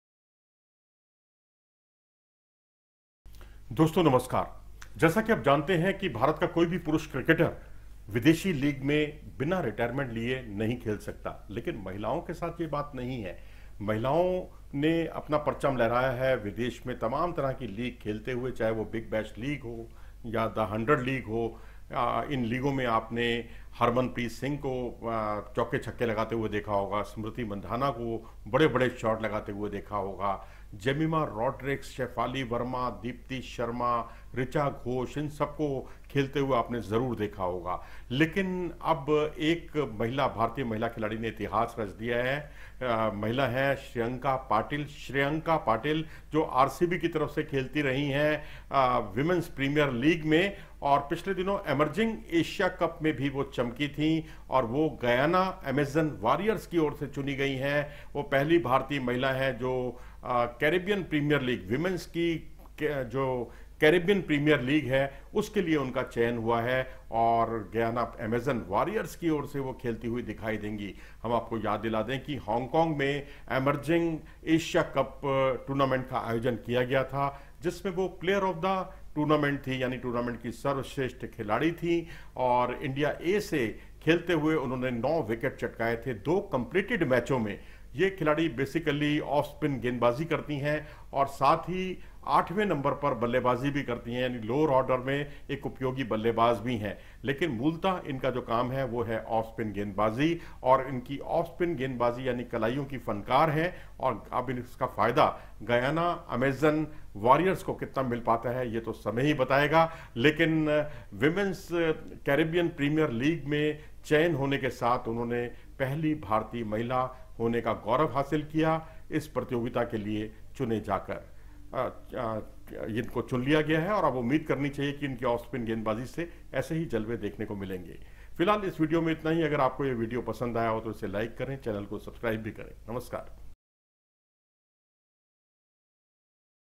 Shreyanka Patil creates history, RCB की इस खिलाड़ी के नाम दर्ज हुआ एक रिकॉर्ड
Shreyanka Patil has been signed by the Guyana Amazon Warriors for the upcoming Women’s Caribbean Premier League (WCPL). She becomes the first Indian women to play in WCPL.